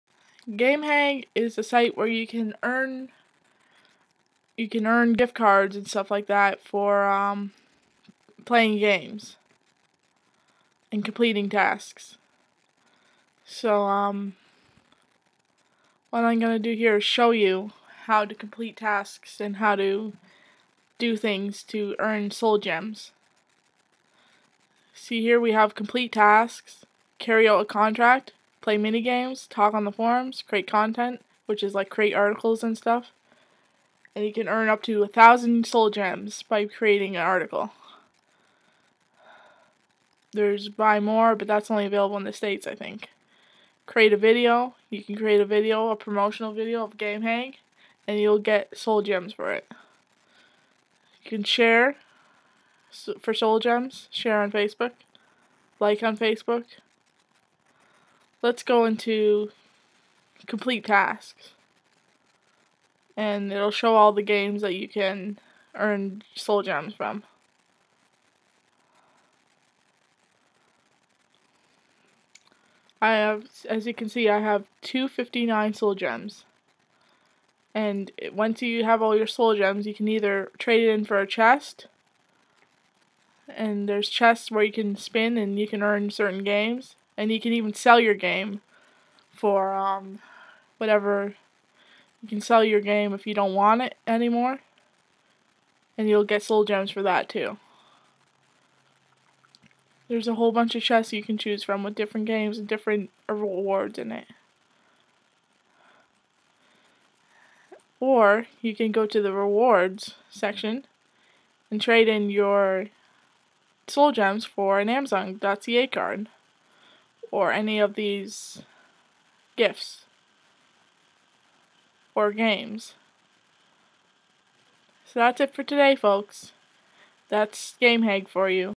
GameHag - Get free games and rewards
GameHag is a site where you can get free gems called soul gems for playing games or completing small tasks and doing surveys. You can trade in your soul gems for virtual items like chests, games and much more.